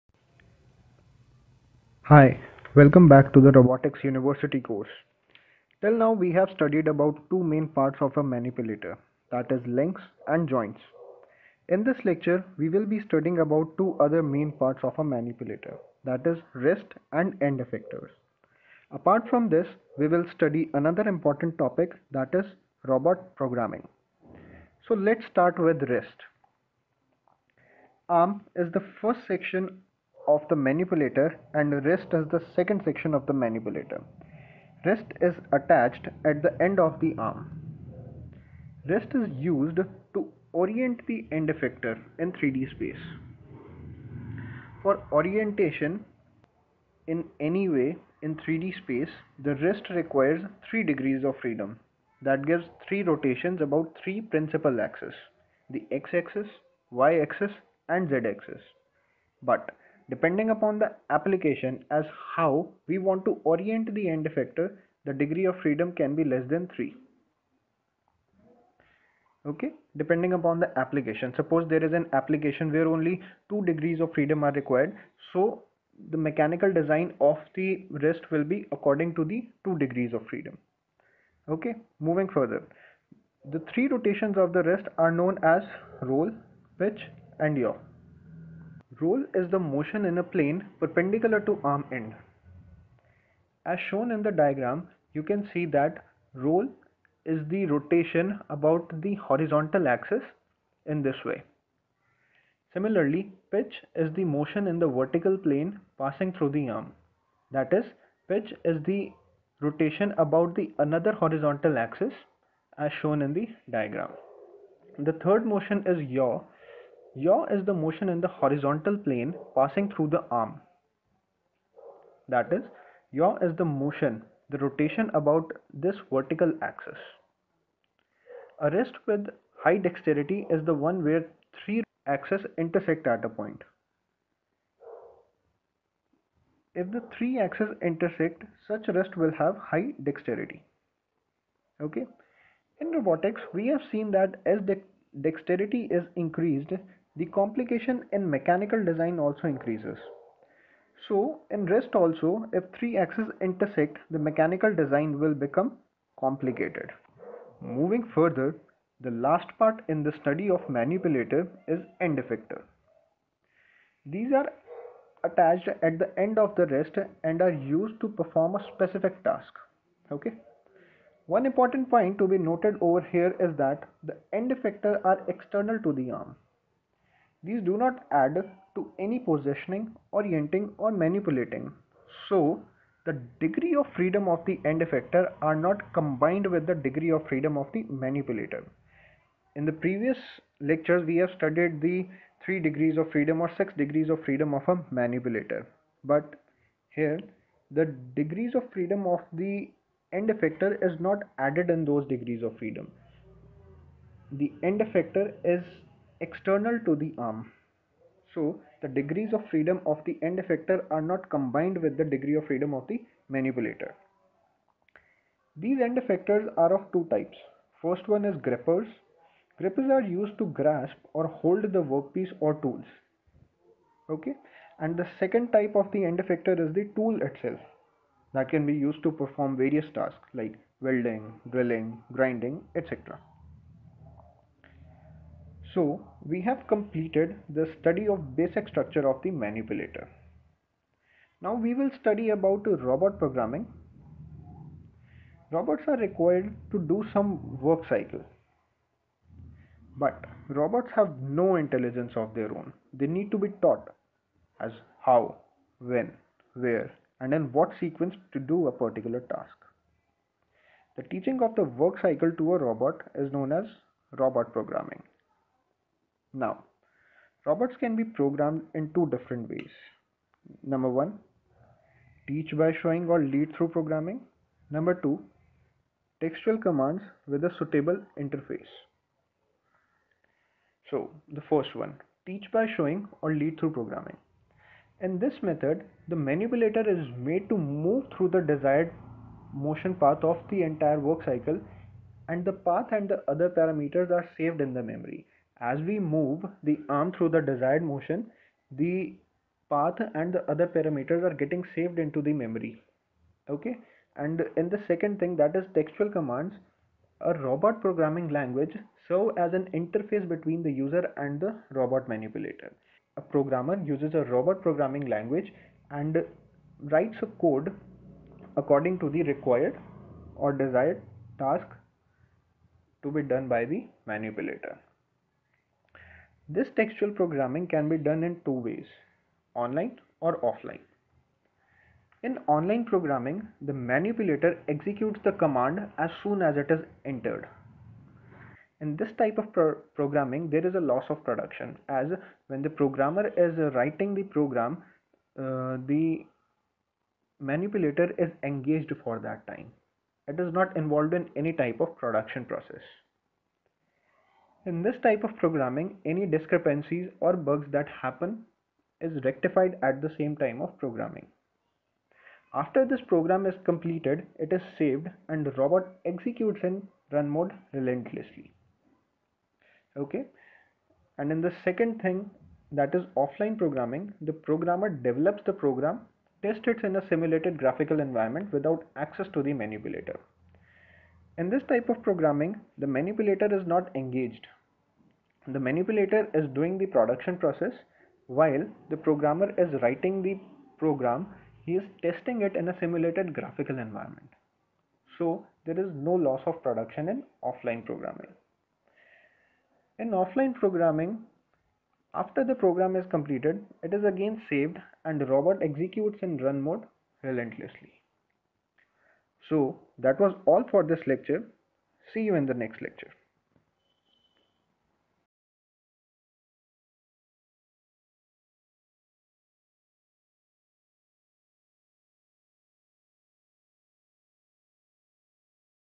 Lecture 5 | Part 1 | Robotics University Course | Wrist, End Effector & Programming
Lecture 5 | Robotics University Course

Topics Covered:
Wrist Configuration
End-Effector
Robot Programming